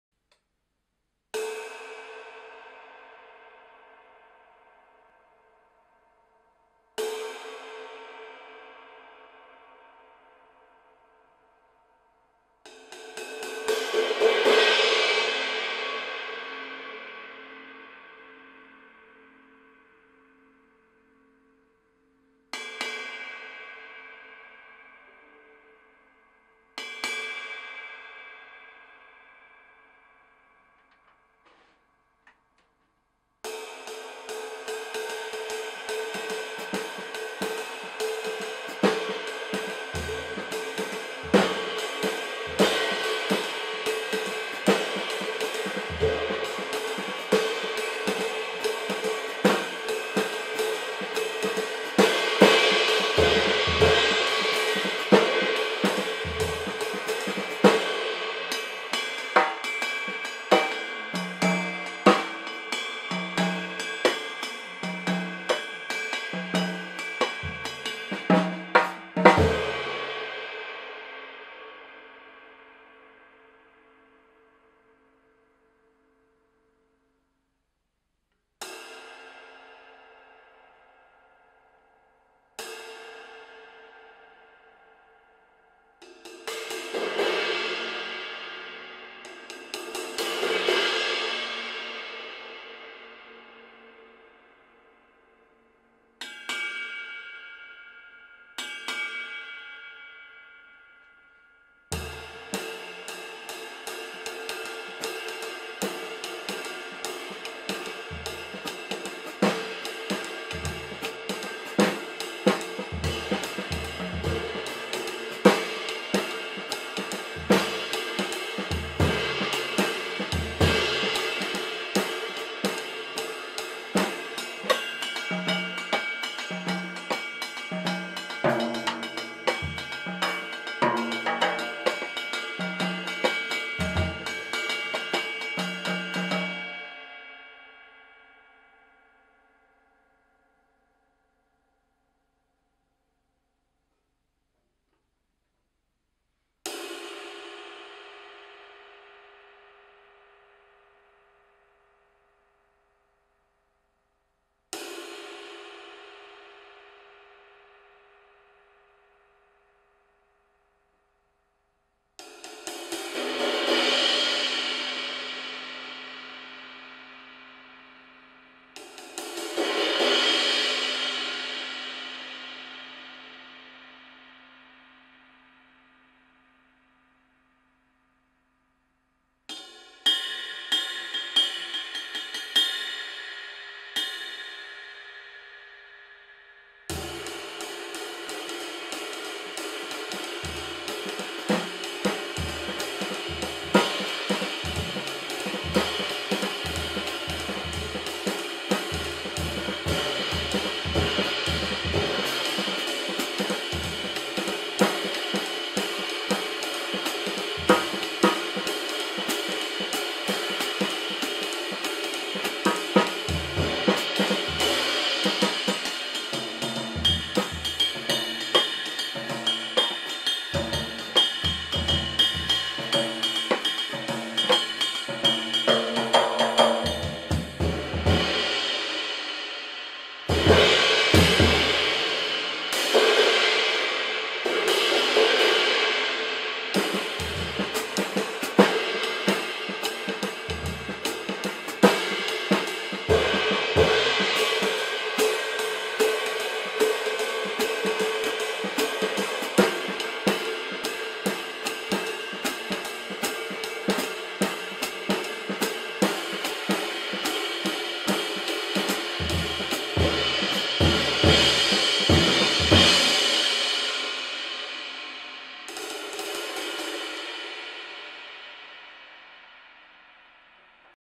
Three Cymbal & Gong 22" cymbals
With apologies for the out of focus video, these are three new cymbals by Portland, Oregon company Cymbal & Gong, made in Turkey.

From left to right:
-- 22" Holy Grail Ride - "Louis"
-- 22" Holy Grail Ride - "Richard"
-- 22" Leon Collection Crash - "Juan"

I don't know the gram weights, but the HG cymbals are a little heavier than normal "jazz ride" category. The LC is roughly a medium thin crash.

If you have questions about Cymbal & Gong cymbals, or if you want to inquire about buying any of these cymbals, and hit the EMAIL TODD link.